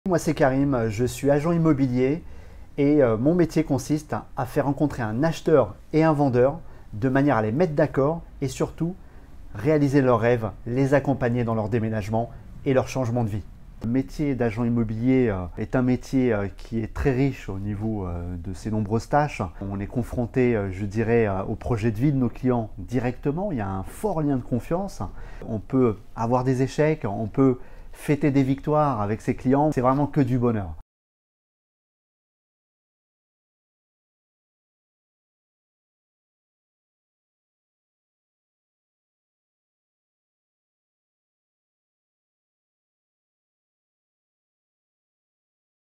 Devenir agent immobilier : mission, salaire, études
Missions, études, salaire d’un agent immobilier… Karim te partage les différentes facettes du métier : de la prospection de biens à l’organisation de visites, en passant par la négociation et la communication... Si tu galères dans ton orientation professionnelle, passe une semaine avec un agent immobilier !

T’es intéressé·e par les métiers de l’immobilier ? Tu souhaites devenir agent·e immobilier·ère ?